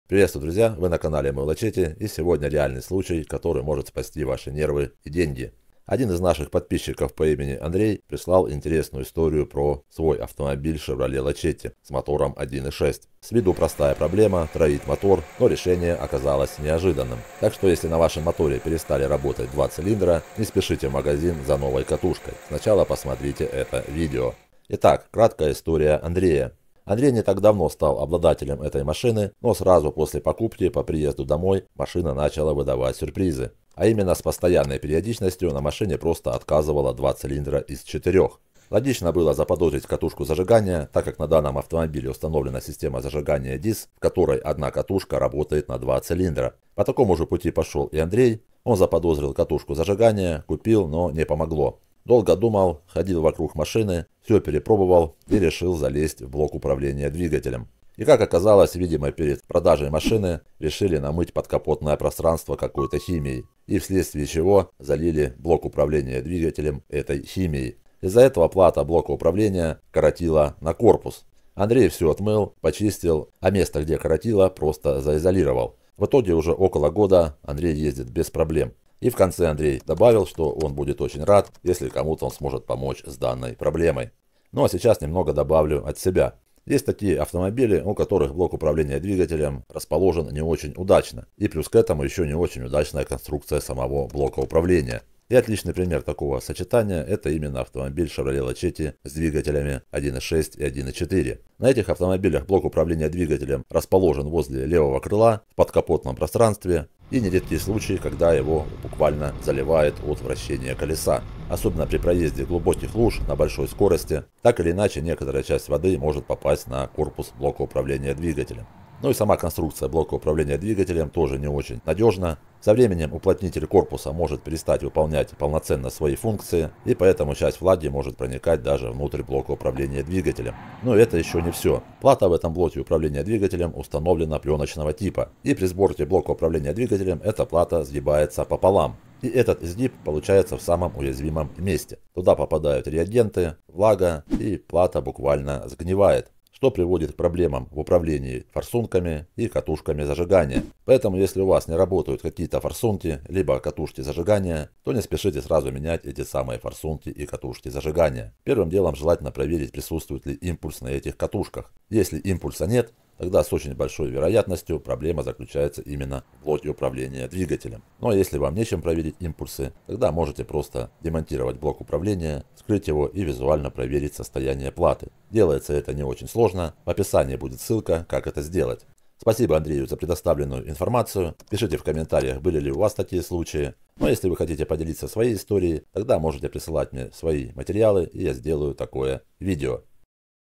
Двигатель Троит — в чём была НАСТОЯЩАЯ причина?
Приветствую, Друзья🖐
После покупки автомобиля начались неожиданные проблемы — двигатель начал троить, периодически переставали работать два цилиндра. Сначала Андрей подумал на катушку зажигания, заменил — не помогло. Перепробовал всё, и только когда заглянул в ЭБУ (мозги), нашёл настоящую причину: химия после мойки попала на плату и вызвала короткое замыкание.

✅ Если твой автомобиль троит — возможно, причина такая же.